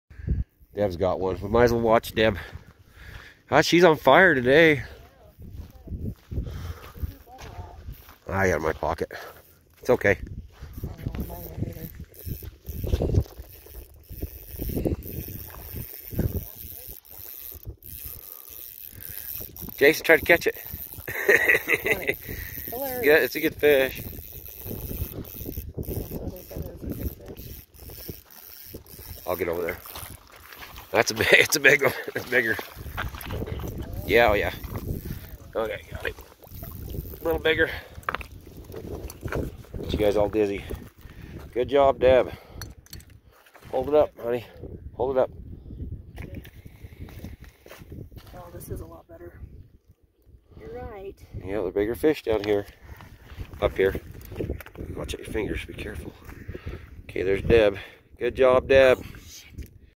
more fishing. panguich lake is slow, but still worth it!
we had a somewhat successful trip. the fish are not vary hungry, due to the mayfly hatch. plane on spending time, to catch anything!